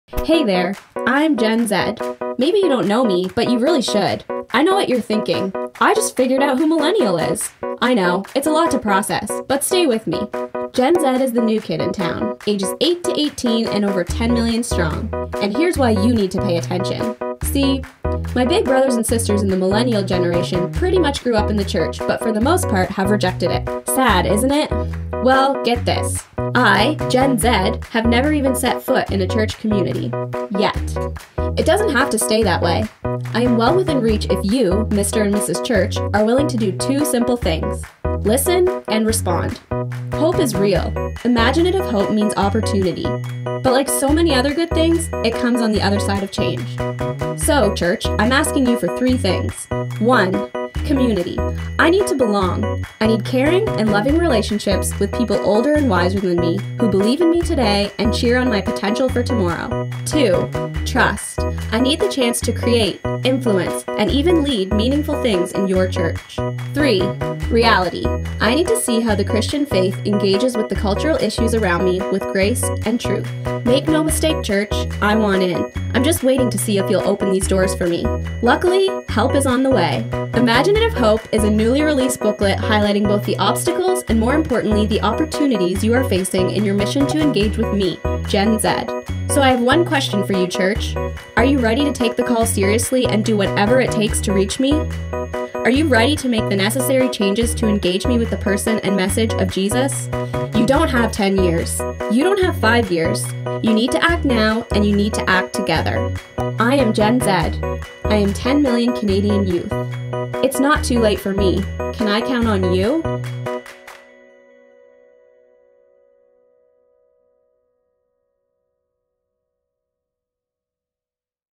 Imaginative Hope
50 Canadian Baptist youth pastors offer these 5 opportunities and 3 action steps to all Canadian Baptist churches.